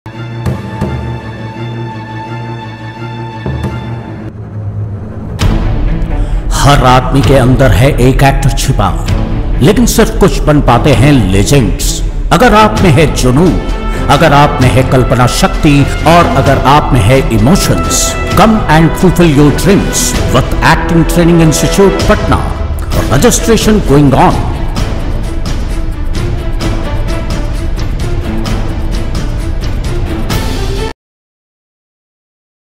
Acting Training institute Patna
Run by Lotus Avio and Deepayan
15 days crash course in acting
topics
Action problem.Improvisation.Acting technique.technique.sceance study.voice and speech diction.Acting and screen testing.